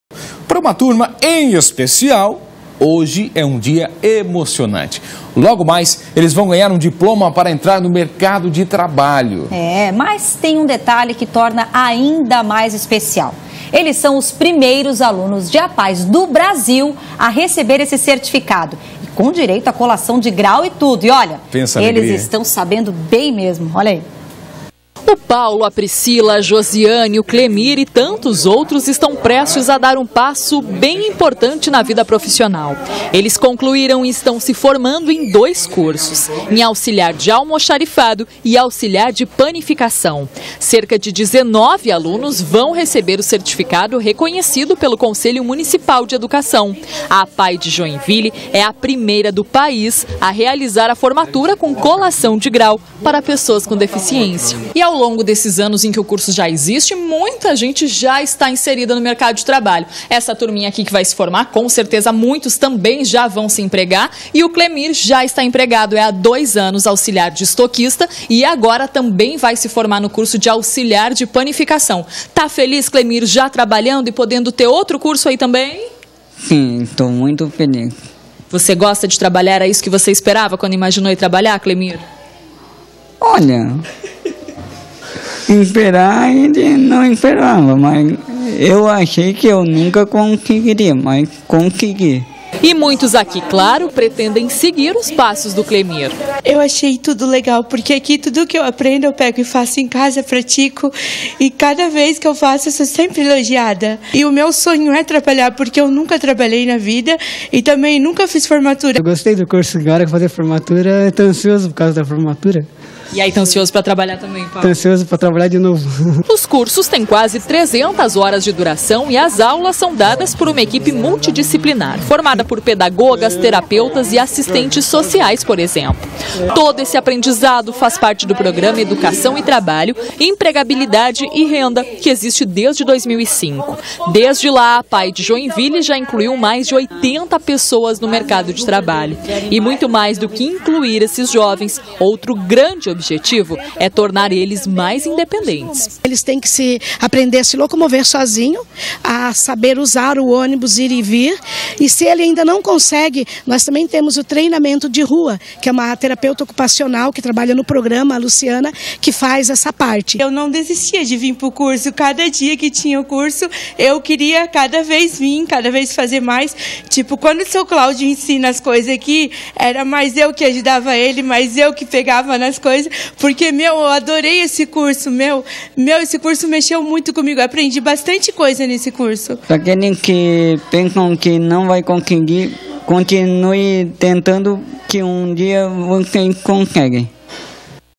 Apae de Joinville é a primeira do país a formar alunos em curso profissionalizante
Para uma turma em especial, hoje (9) é um dia emocionante. Eles vão ganhar um diploma para entrar no mercado de trabalho. E tem um detalhe que torna ainda mais especial: eles são os primeiros alunos de Apae's do Brasil a receber esse certificado, com direito a colação de grau. Confira!